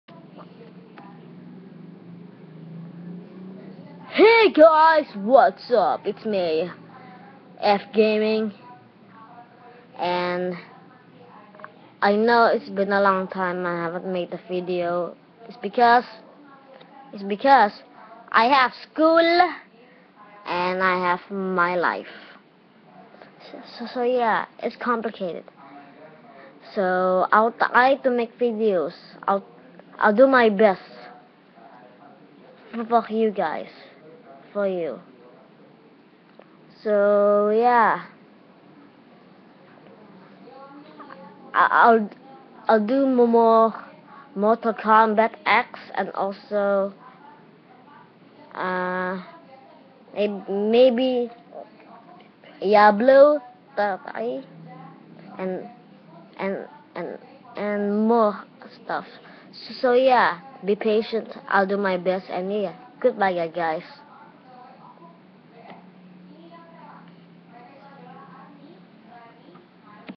update video
This is my official face reveal
No i'm not a kid
I'm 14
Also
my voice is different because my mic is broken so i will try to fix my mic and i will surely make more videos school and also my life is the problem i will do my best to create more videos for you guys and girls so yeah hope you understand :)